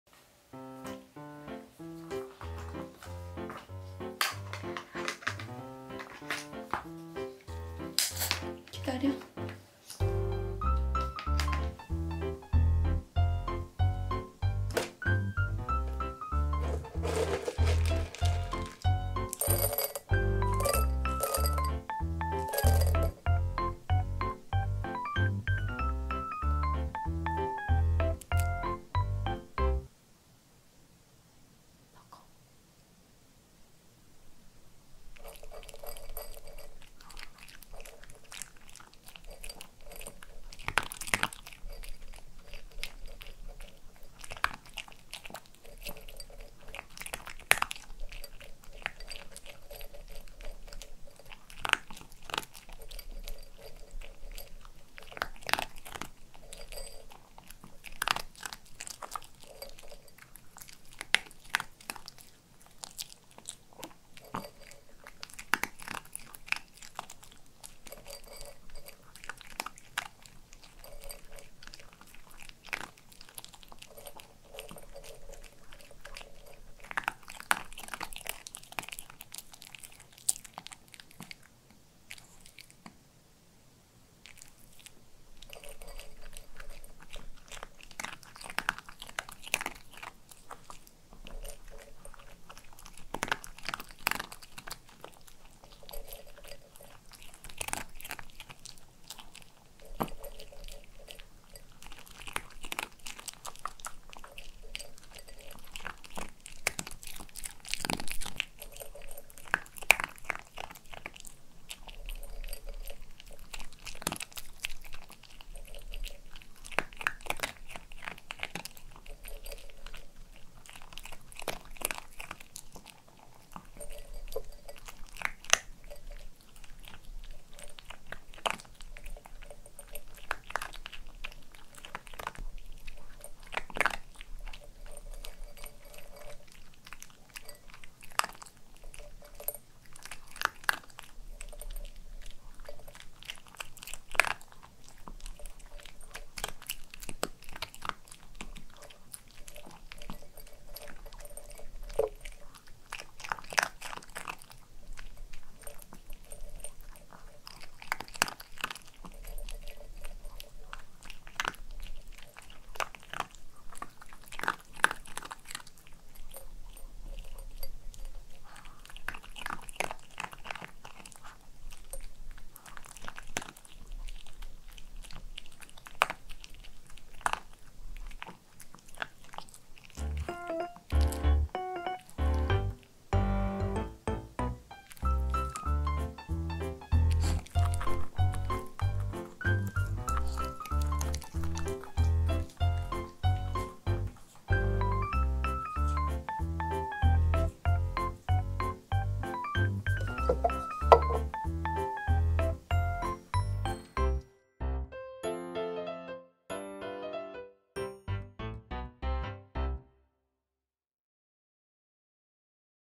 앉은 자리에서 밥 한 그릇 뚝딱 해치우는 강아지 먹방🐶 (오독오독 강아지 사료 먹는 소리) eatingASMR , CRUNCHY SOUNDS
오늘은 제가 개인적으로 좋아하는 설이의 사료 먹방 영상입니다!
오독오독  씹는 소리가 정말 좋아요:)
그럼 즐겁게 시청해주세요.

Profile💛
Name : Hong seol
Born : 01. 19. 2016
Breed : White Pomeranian(♂ )
Favorite Food : Yogurt&Cheese